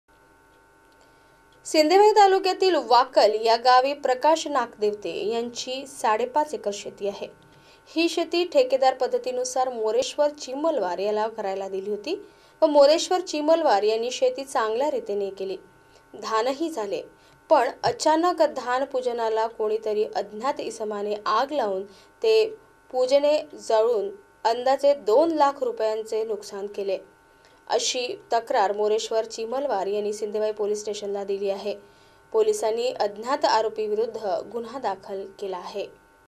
22 November 2018 Search Tv Chandrapur - साडेपाच एकर धानाचे पुजने
साडेपाच एकर धानाचे पुजने आगीत जळून खाक